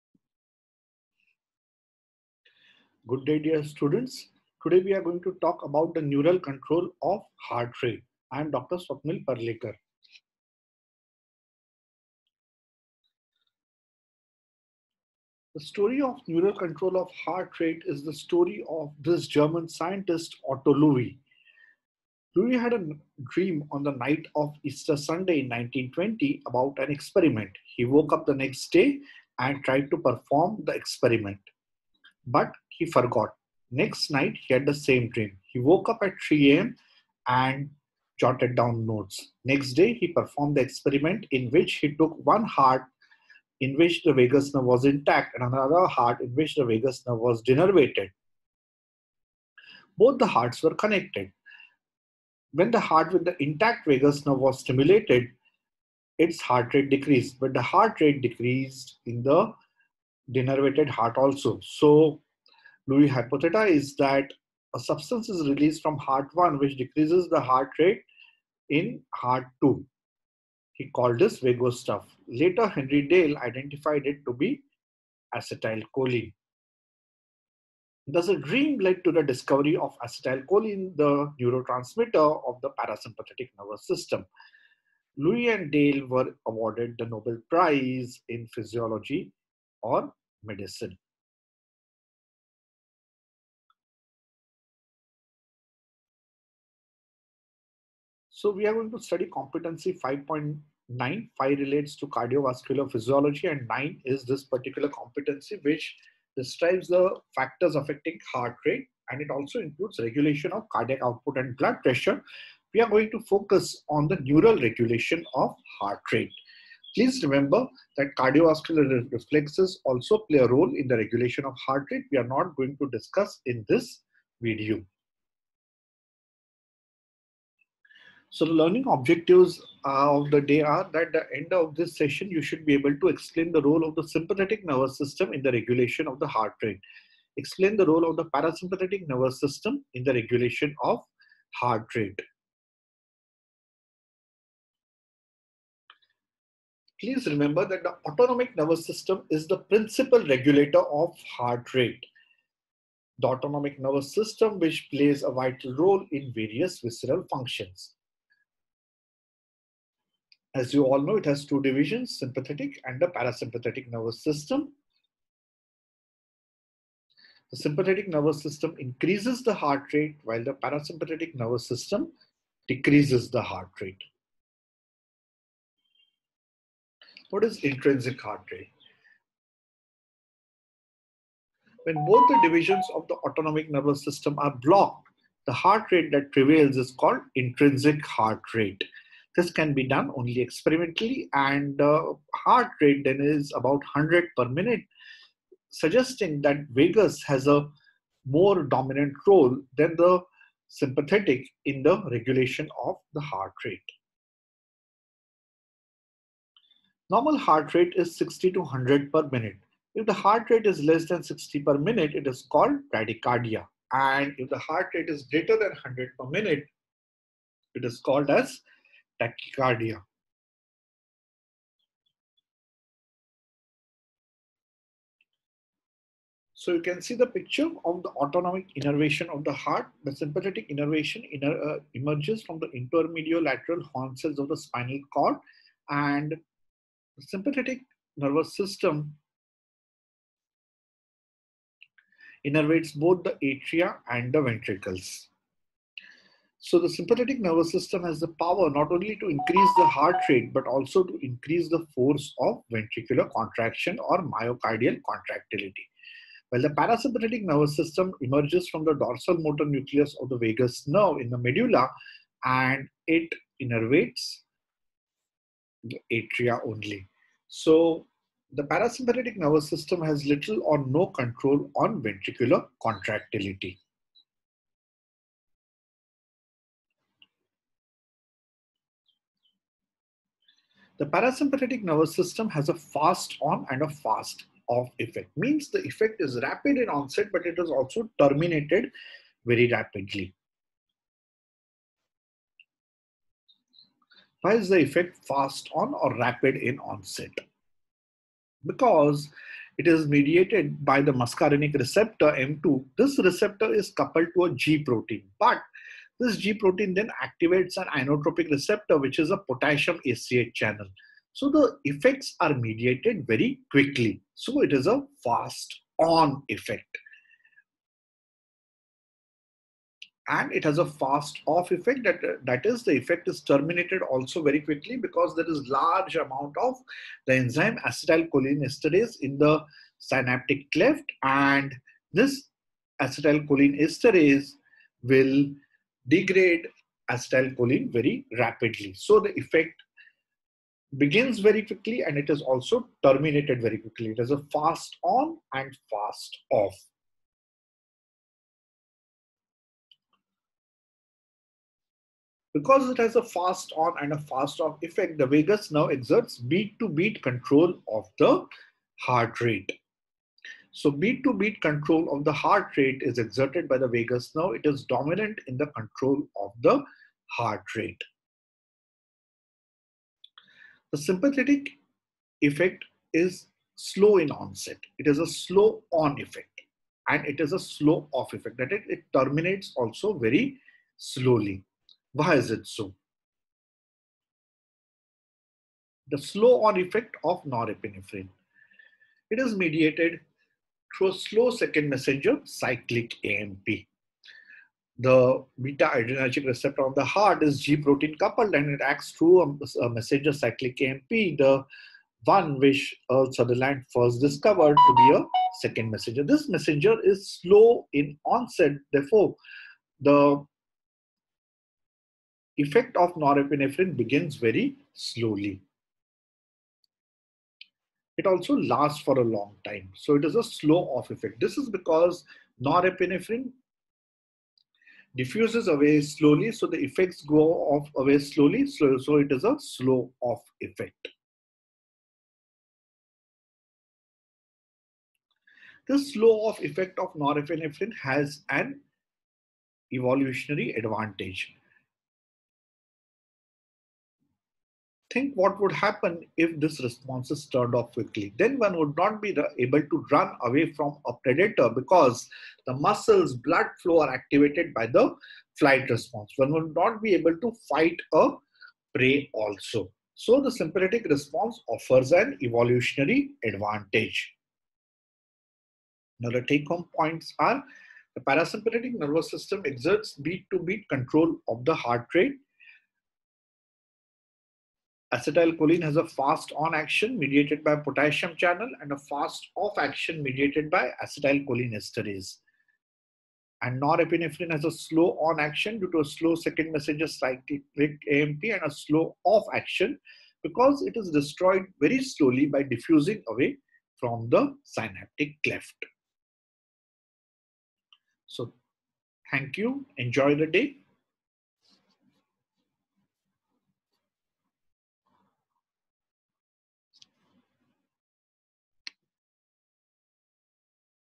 Heart Rate
A detailed explanation of the role of the autonomic nervous system in the regulation of heart rate